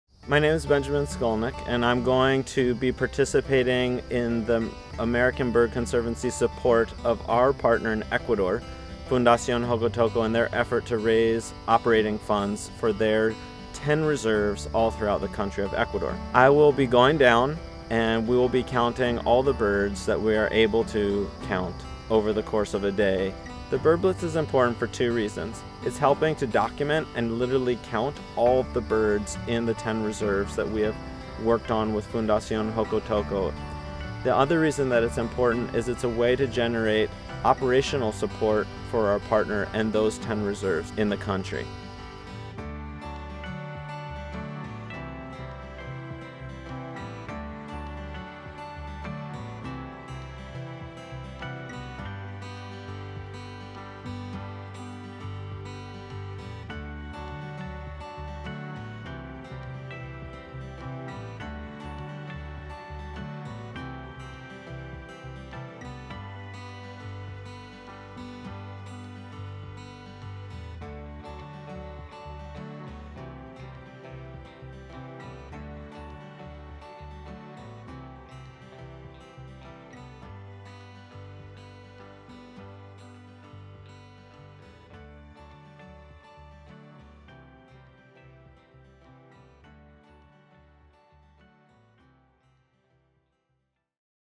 Bird Blitz 2014 - Ecuador
ABC's International Conservation Officer, Benjamin Skolnik, explains why supporting our international partner, Fundación Jocotoco, during the 2014 Bird Blitz is important for bird conservation in Ecuador.

To learn more about the BirdBlitz and make a pledge,

Additional photos courtesy of Fundación Jocotoco and Benjamin Skolnik.